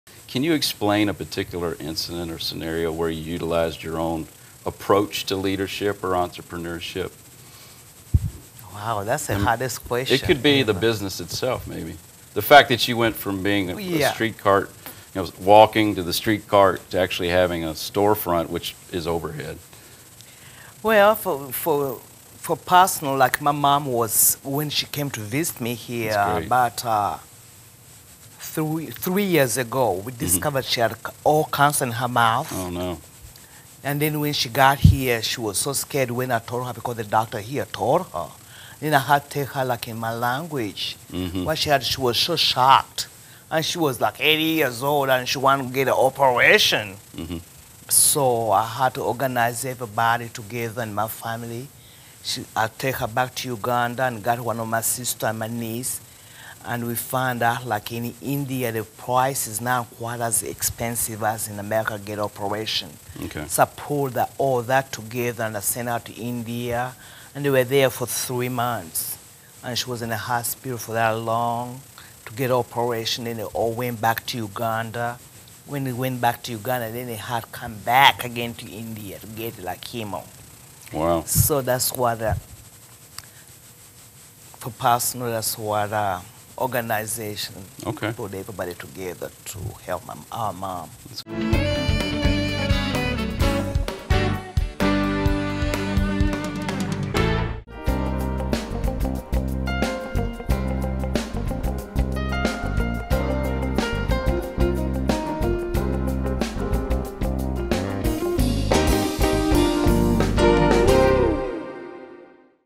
🍪🍪 Leadership-Elizabeth Kizito-Fdr-Kizito’s Cookies 🍪🍪
Episode 148. Elizabeth Kizito - Owner & Founder, Kizito’s Cookies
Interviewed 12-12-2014

ABOUT ELIZABETH KIZITO

Elizabeth Kitzito sees her cookie business as a way to provide for her family here & in Uganda.

Born in Uganda during the 1950s, Kizito moved to America in 1975 to attend school. Unfortunately, civil unrest & war in Uganda prevented her from returning. She moved to Louisville in 1978 & began making & selling cookies – originally as a way to pay for her son’s birthday present. She started selling cookies downtown & within a few years, in 1989, she traded her cart for her own bakery on Bardstown Road.